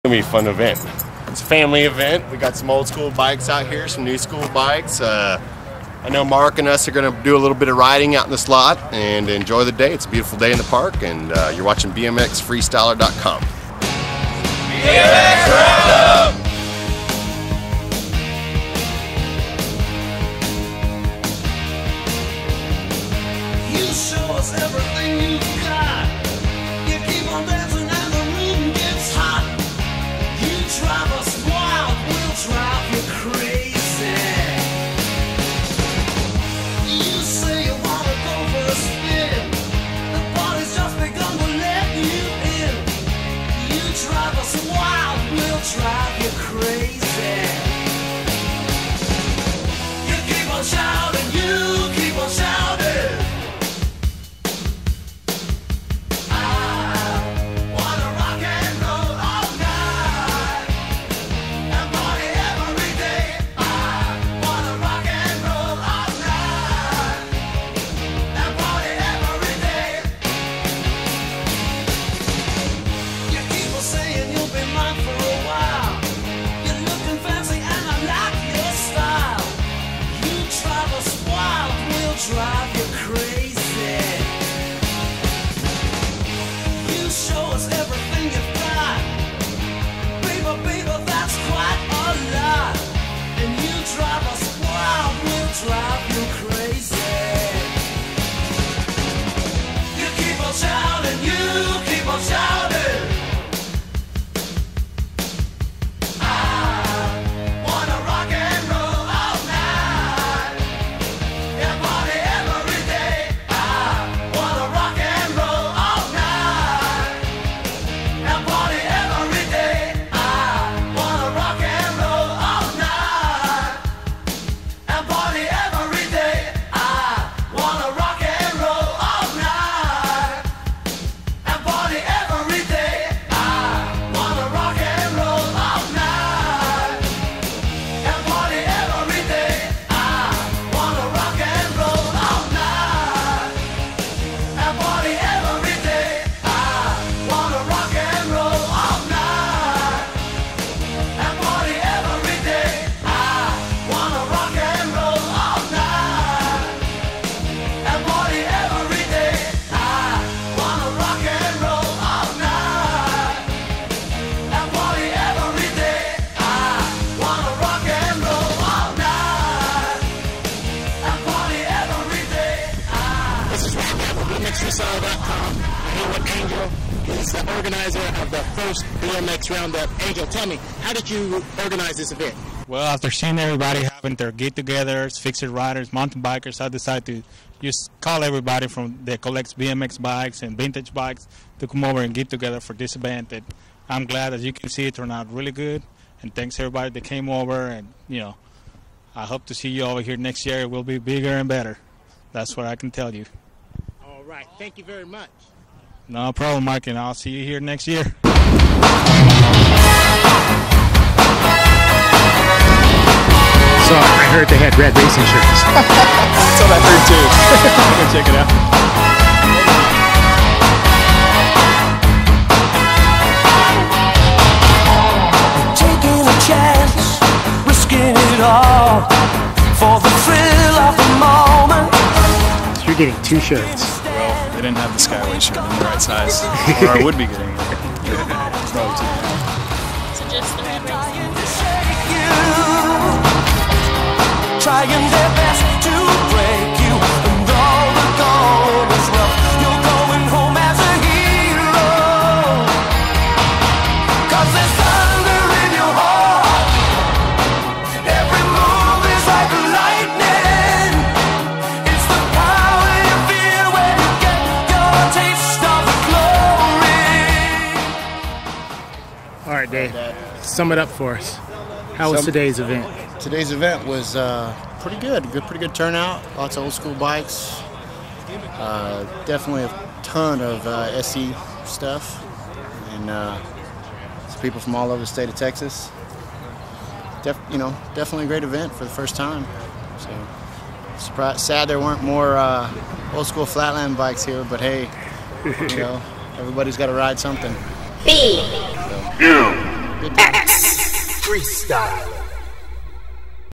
The 1st Annual BMX ROund Up
This was the 1st Annual BMX Round Up from April 25, 2010 held in Zilker Park in Austin,Texas